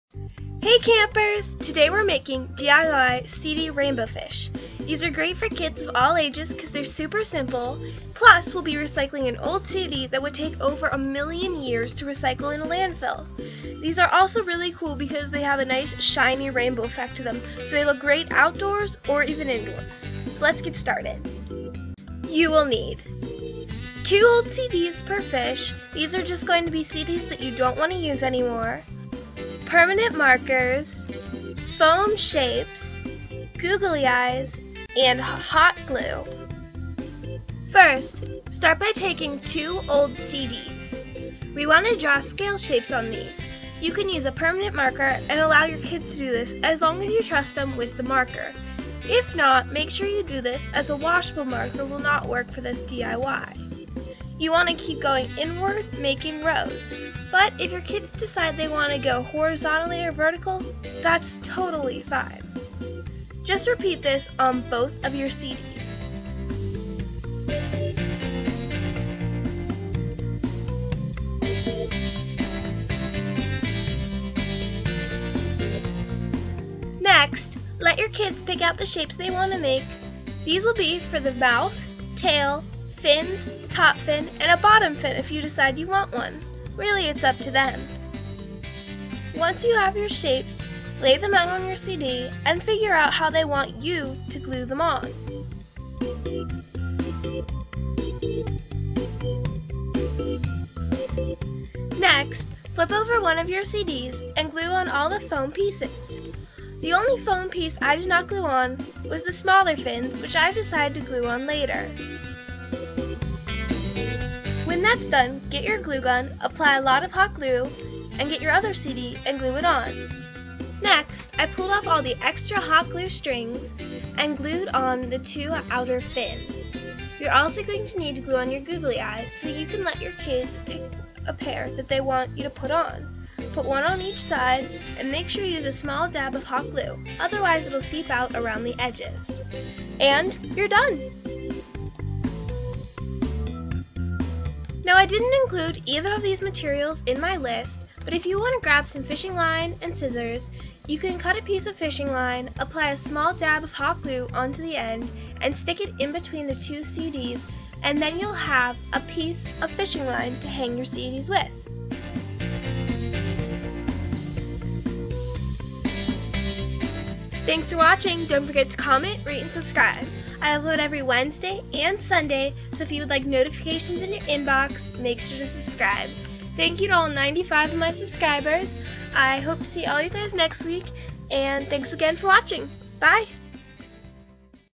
DIY CD Rainbow Fish - Citrus Slices Summer Camp
Hey Campers!

So I said I would upload two videos today, but the video from last week is still not processing. I am going to see if I can re film it some time next moth. If not I still have a lot of fun DIYs coming up!

-Citrus

Materials
-Two CDs
-Permanent Marker
-Foam Shapes
-Google Eyes
-Hot Glue